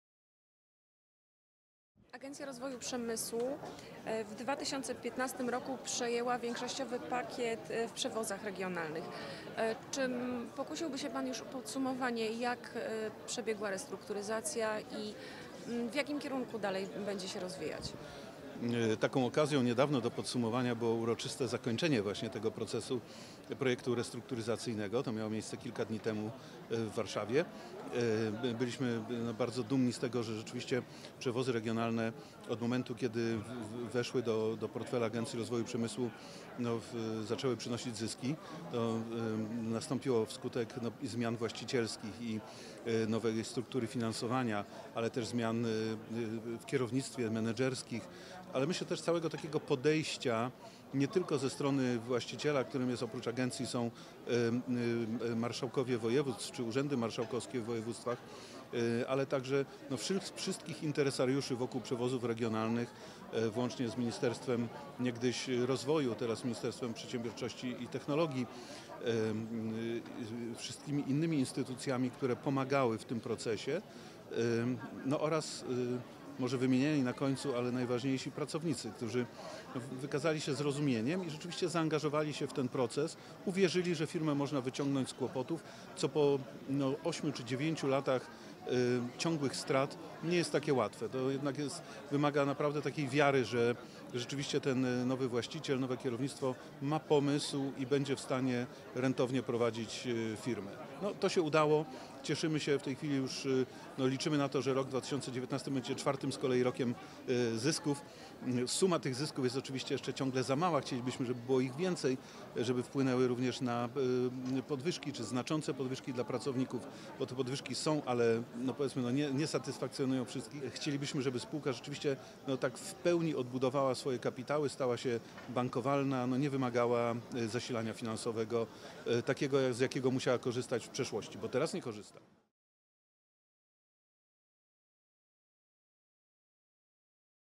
Restrukturyzacja POLREGIO przynosi efekty. Przewozy regionalne zaczęły zarabiać
Przewozy regionalne od czasu, gdy trafiły do portfela Agencji Rozwoju Przemysłu zaczęły przynosić zyski - mówił ISBnews.TV Andrzej Kensbok, wiceprezes Agencji Rozwoju Przemysłu.